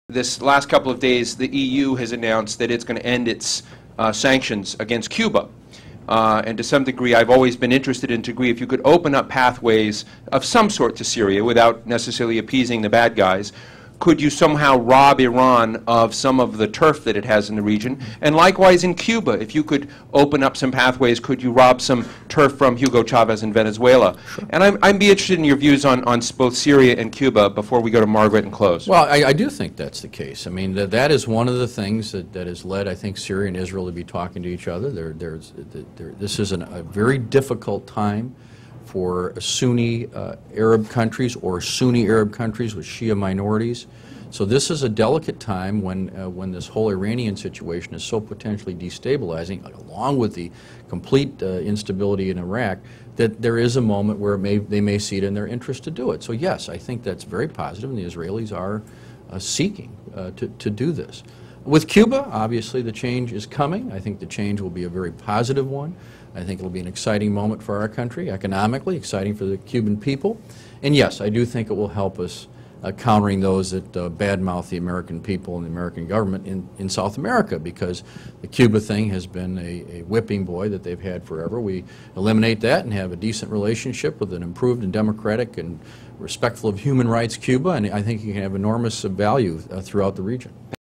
Russ Feingold on US-Cuba Policy
Russ Feingold discusses the EU's releasing of Cuba from trade sanctions and what America's next president should do concerning Cuba.